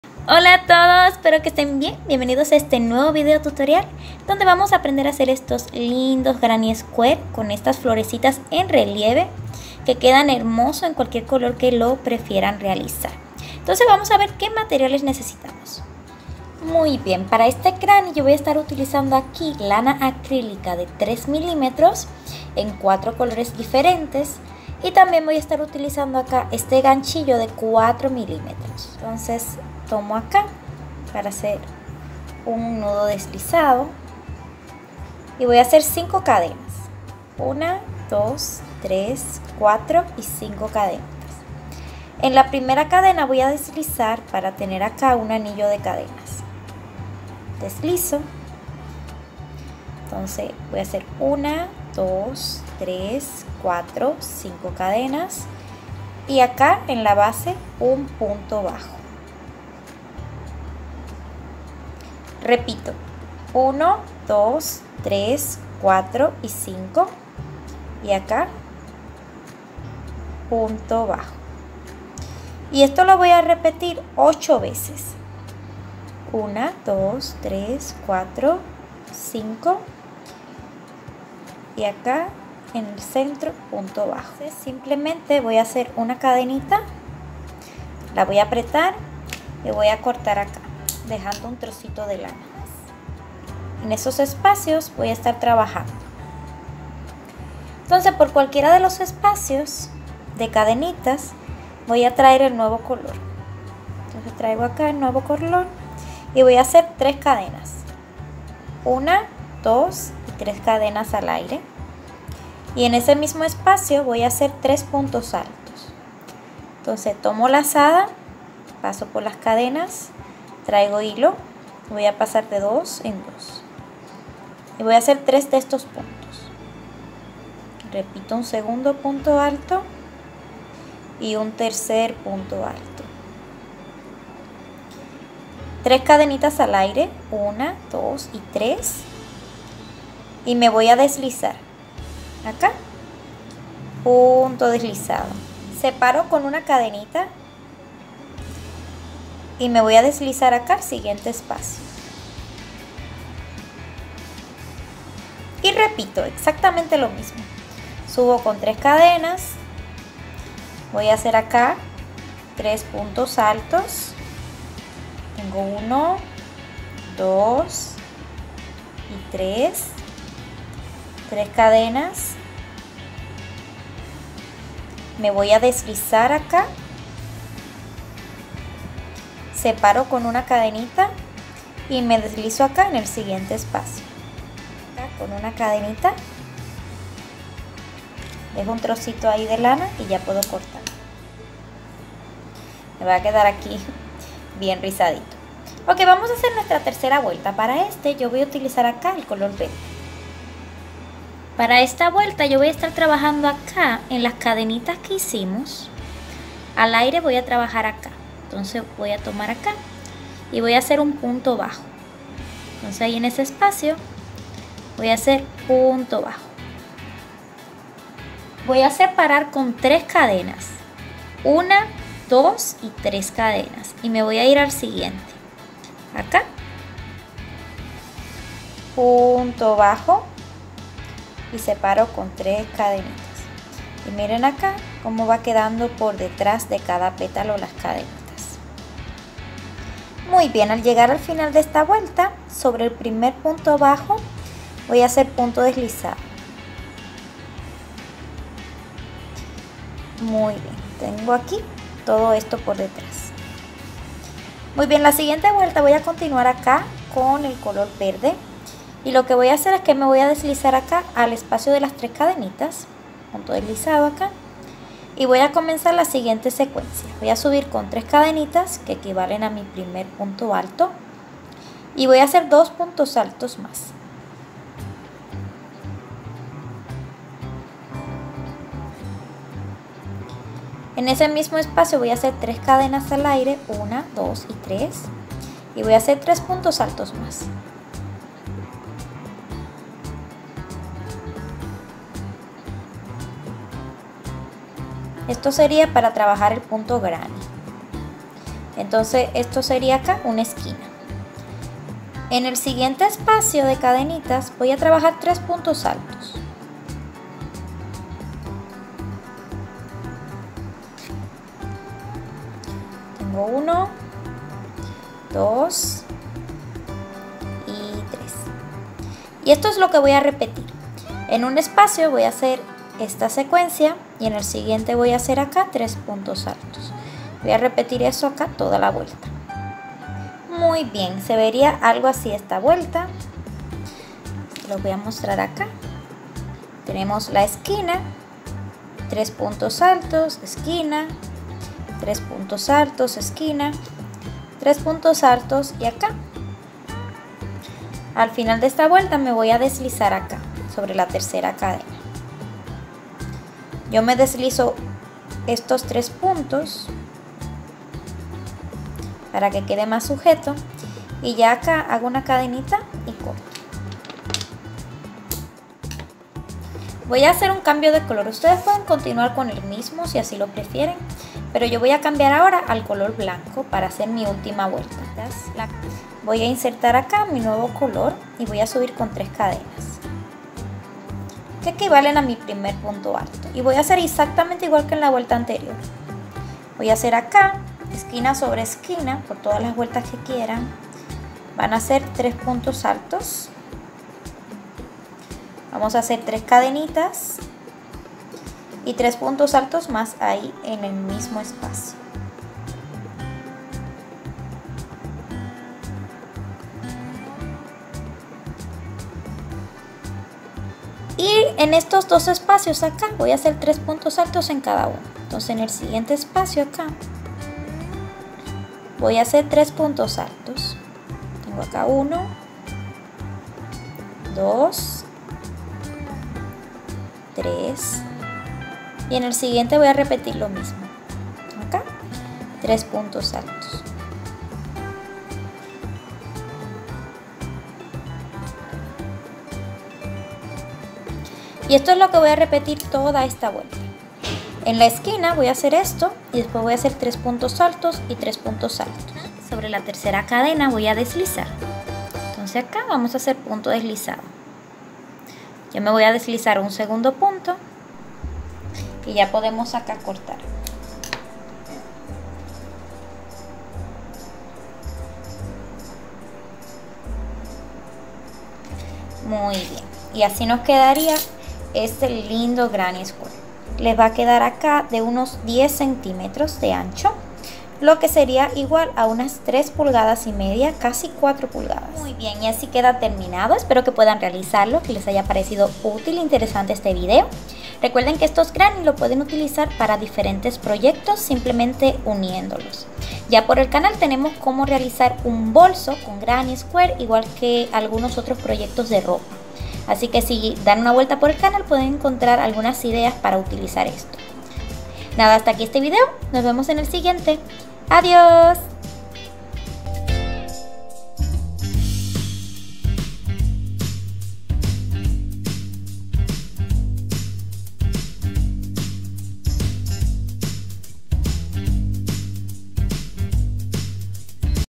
Como Tejer cuadrado Flores a croché paso a paso/ Granny square crochet tutorial/ Cuadrado ganchillo
Tutorial paso a paso de como hacer tejer cuadrados de ganchillo para mantas abrigos cárdigan cojines y mucho mas, Granny square tejido en crochet de flores 3d en relieve hermosos para mantas .